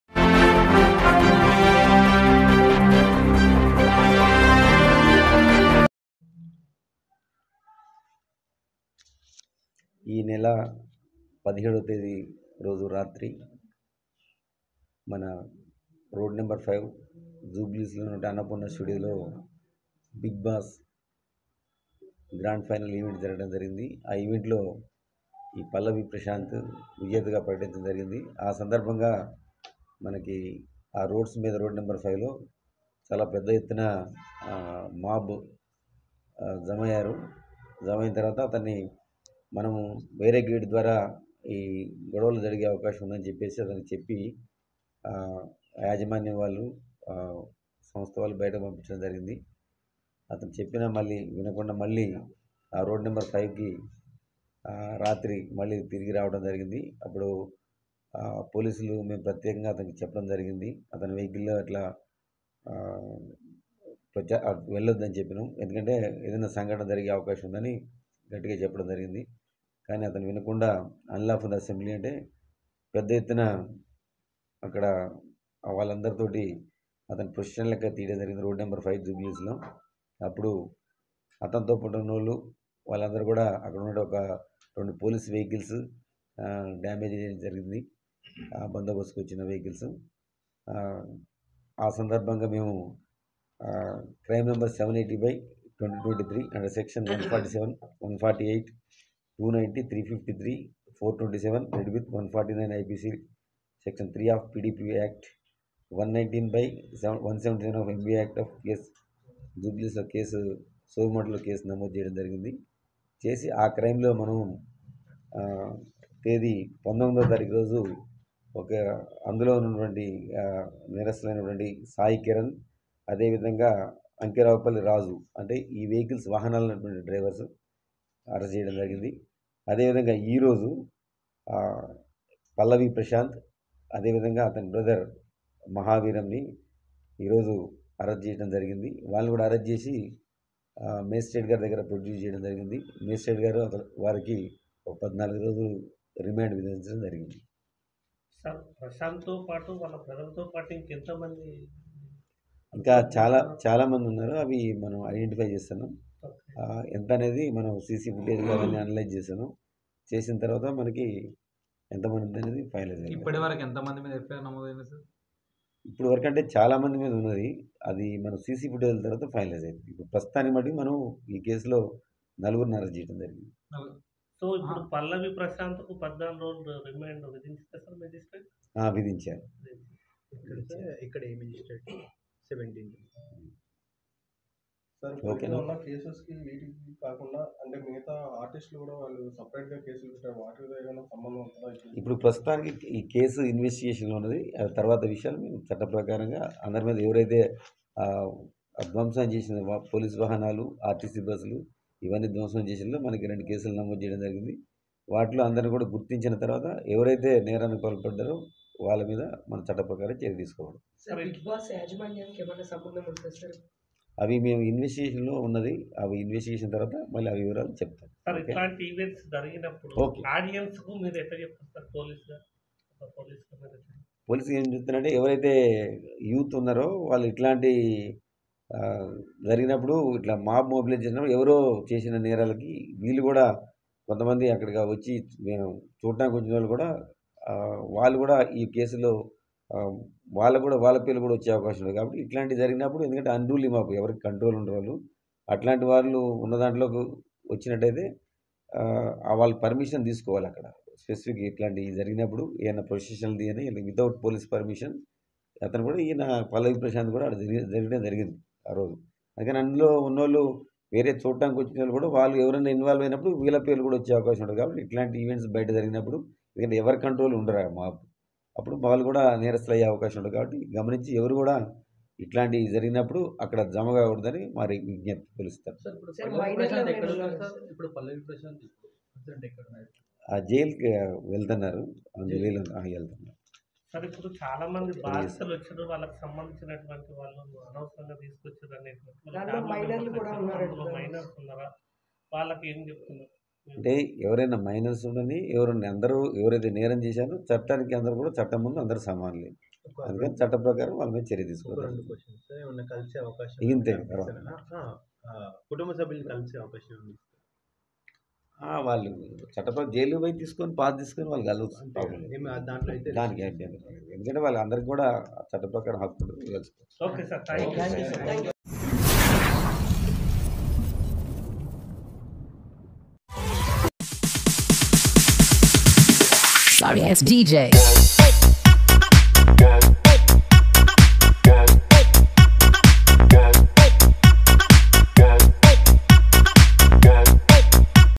BIGBOSS #7 ఛాంపియన్ రైతుబిడ్డ పల్లవి ప్రశాంత్ కు 14రోజులు రిమాండ్
హైదరాబాద్ జూబ్లీహిల్స్ పోలీస్ స్టేషన్ లో  BIGBOSS 7
ఛాంపియన్ రైతుబిడ్డ పల్లవి ప్రశాంత్ కు 14 రోజులు రిమాండ్